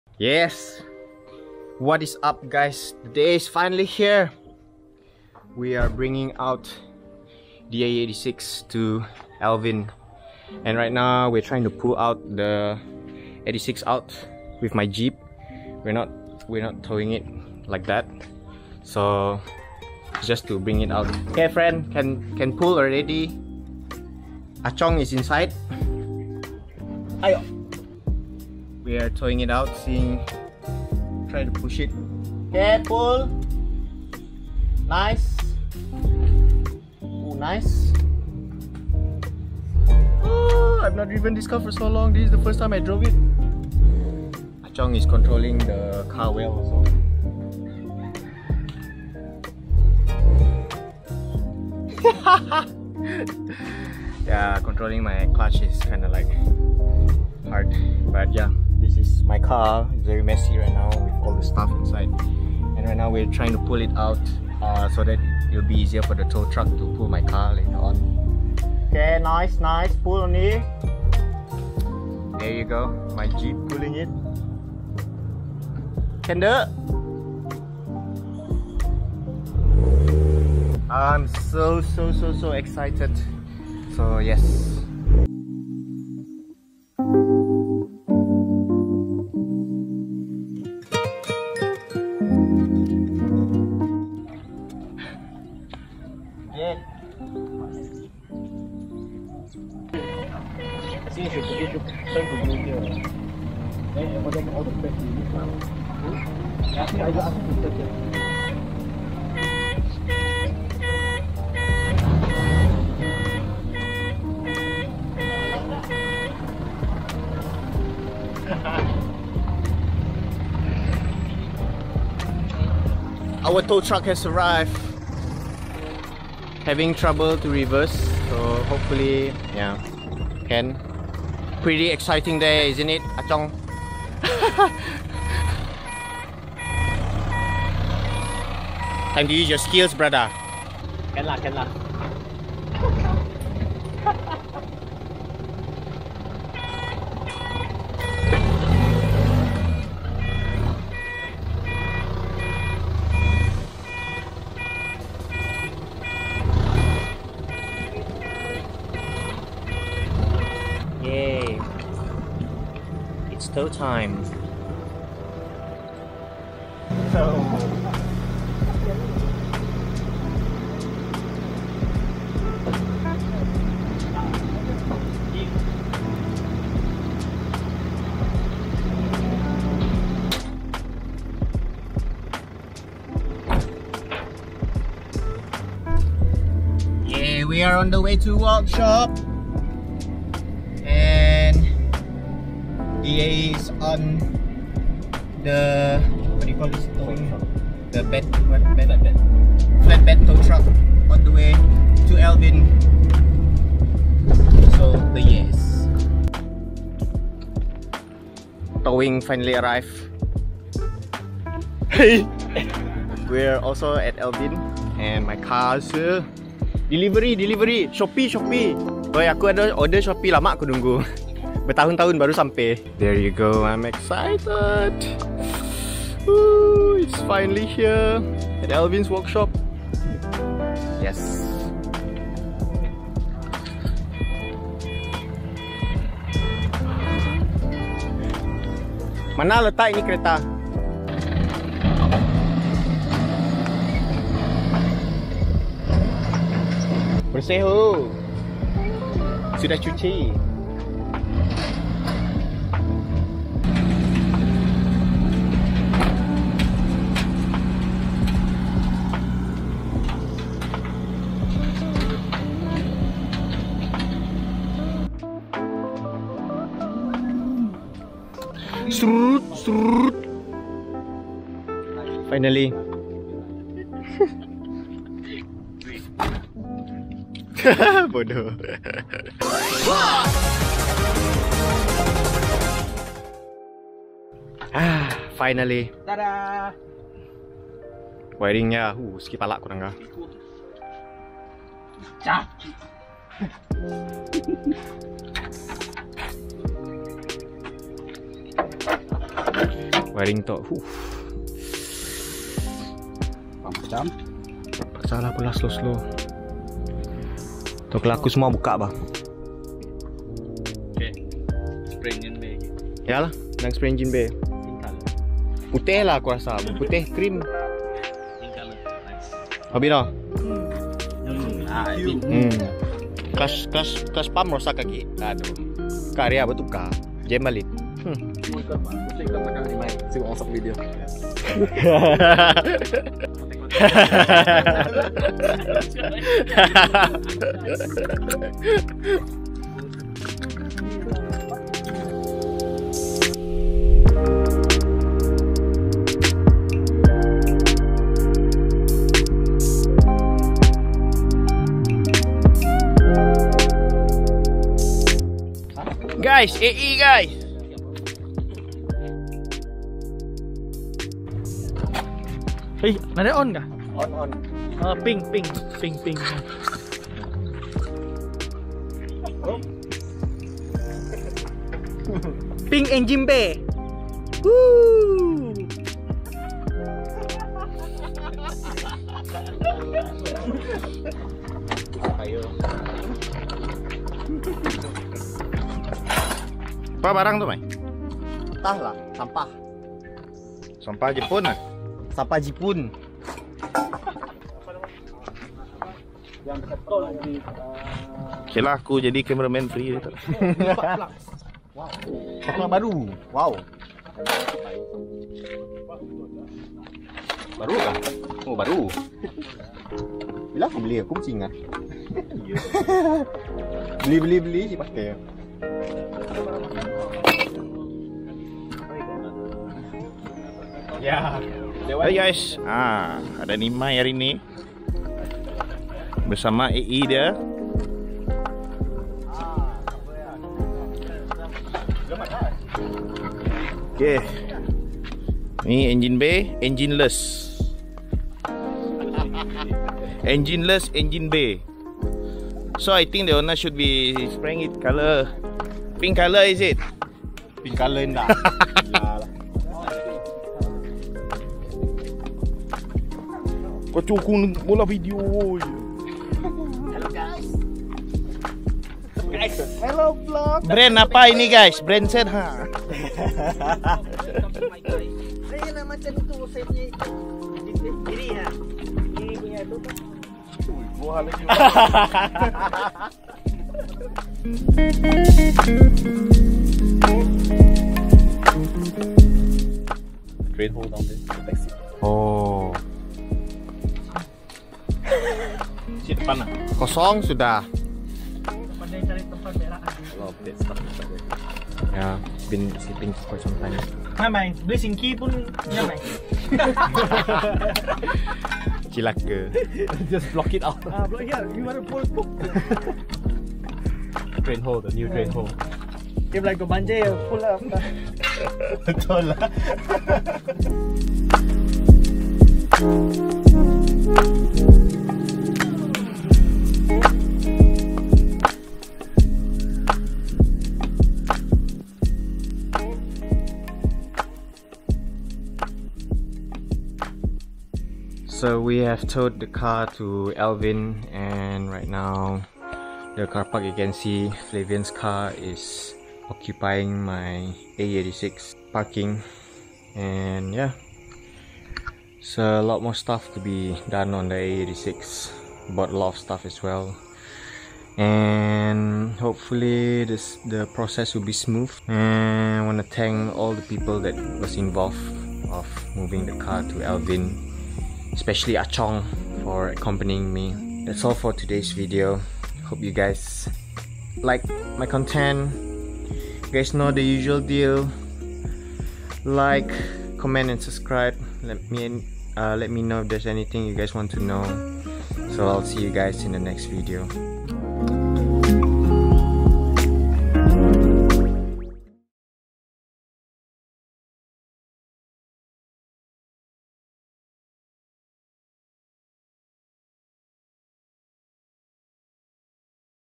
AE86 7AGE Restoration 1 : Finally Getting The Love!
Finally Panda itself has checked in! Crazy excited to start working on it!